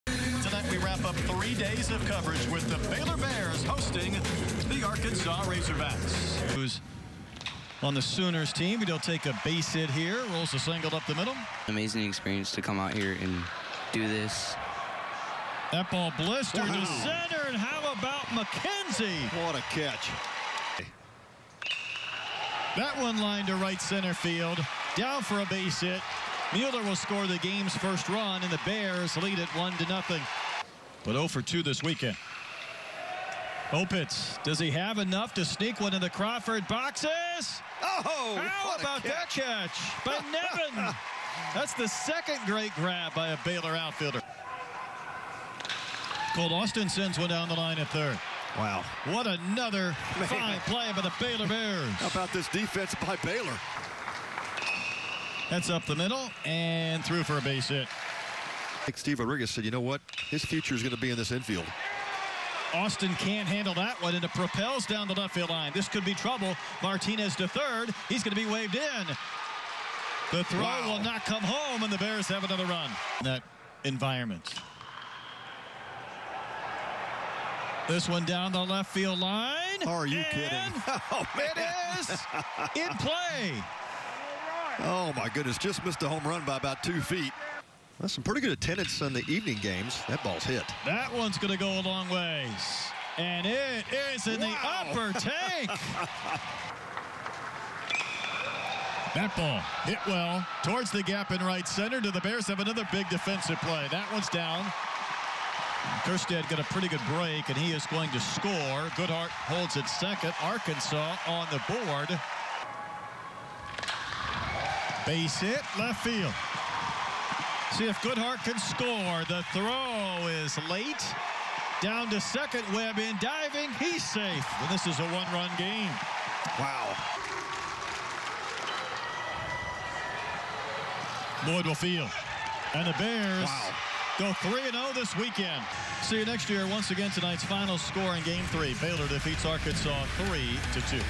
Baylor vs Arkansas Baseball Highlights
Baylor goes 3-0 in the Shiners Hospital College Classic, defeating Arkansas 3-2 on Sunday night.